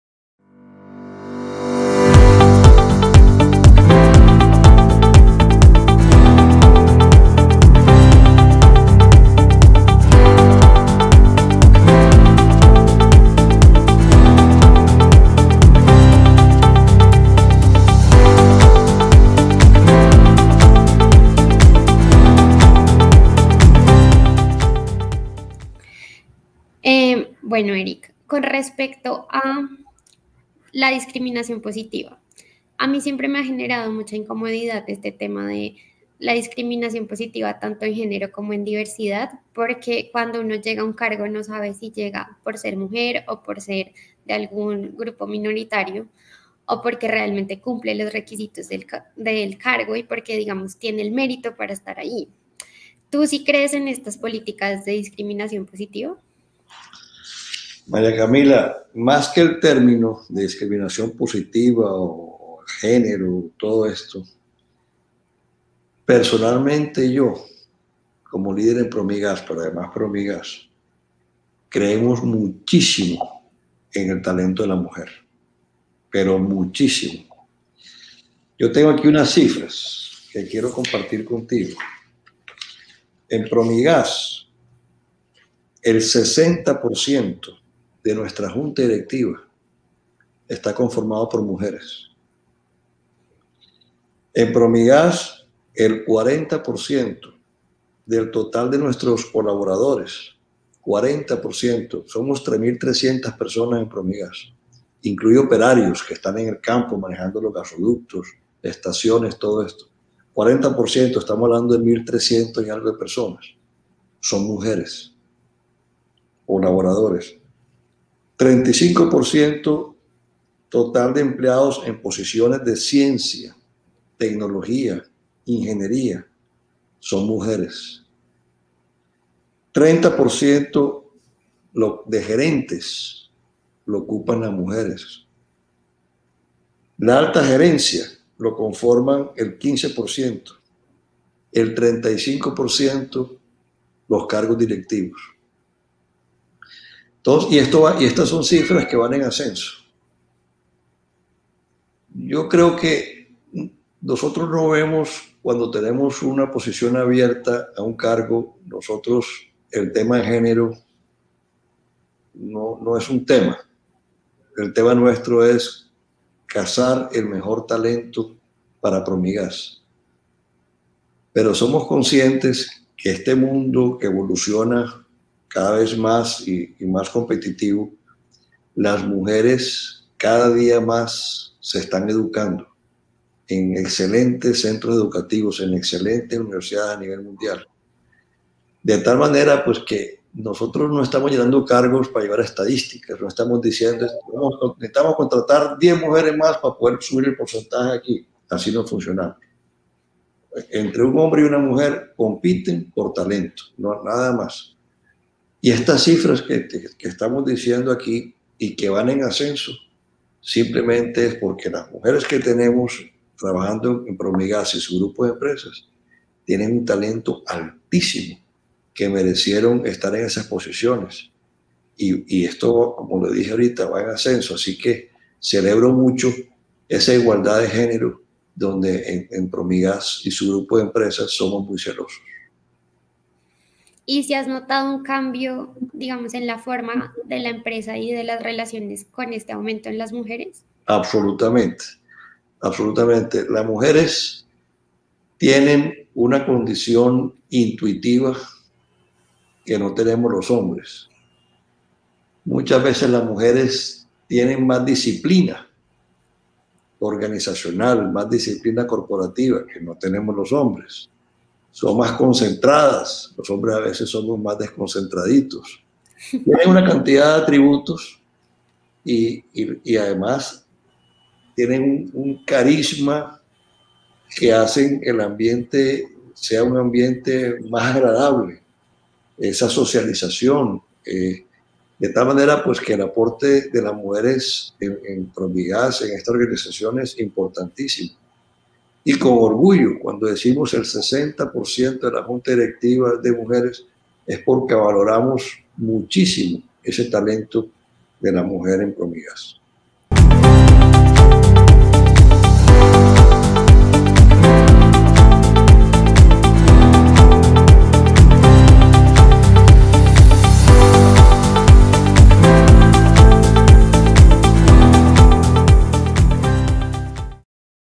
Eric Flesch - Presidente Promigas y sus planes sobre la diversidad de género en Colombia
En la tercera entrega del Especial entre jóvenes y 50 CEO y directivos de las principales empresas en Colombia, Eric Flesch - Presidente Promigas, habló con Maria Camila Forero sobre sus planes sobre la diversidad de género en Colombia